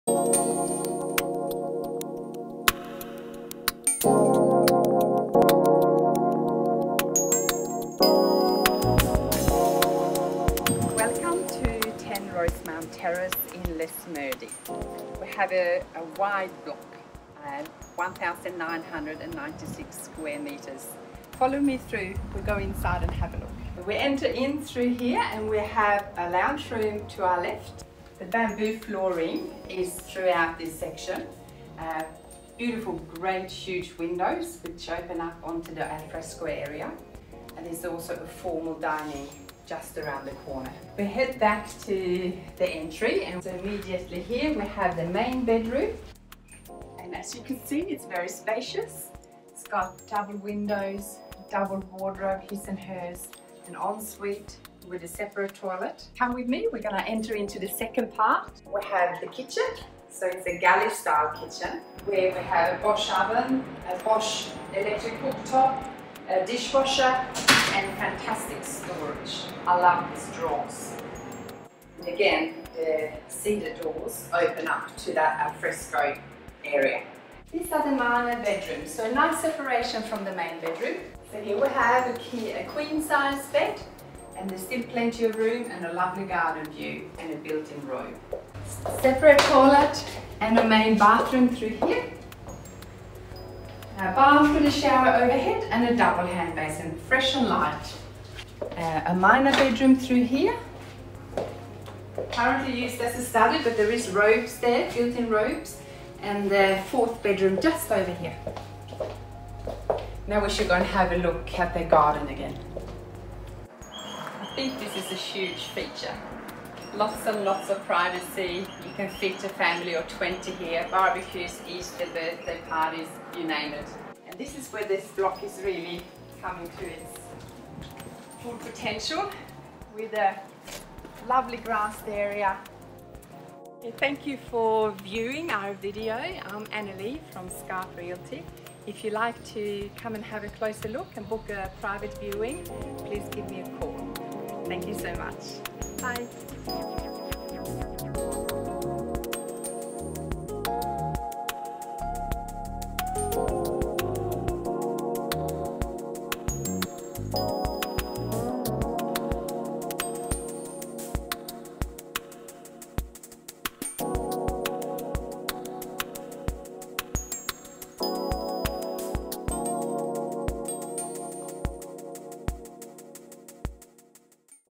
10 Rosemount Terrace, Lesmurdie
A presentation of 10 Rosemount Terrace in Lesmurdie for sale by Scarp Realty Team.
Call Anneli or Susanne to schedule an inspection today!